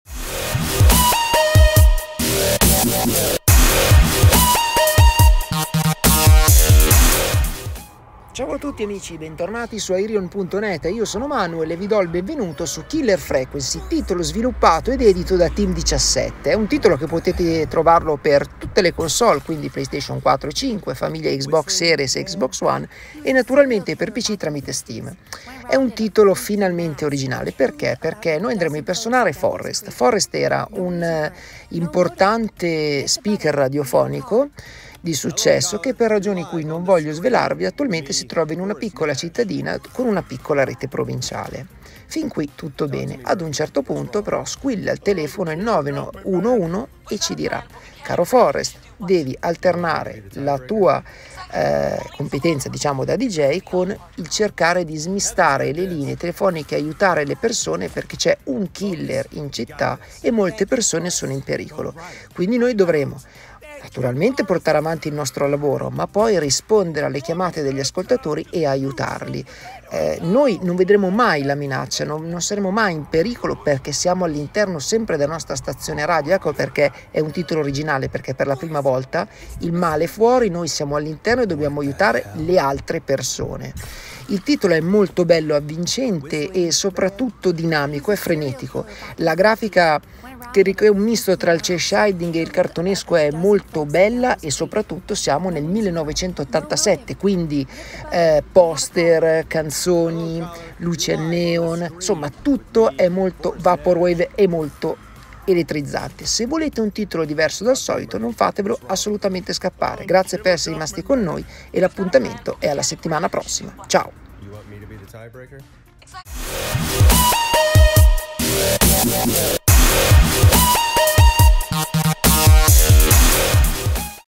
Killer Frequency : la recensione
Una stazione radio del 1987, riceva chiamate di soccorso per un killer nella cittadina. Sarete pronti ad aiutare i vostri ascoltatori?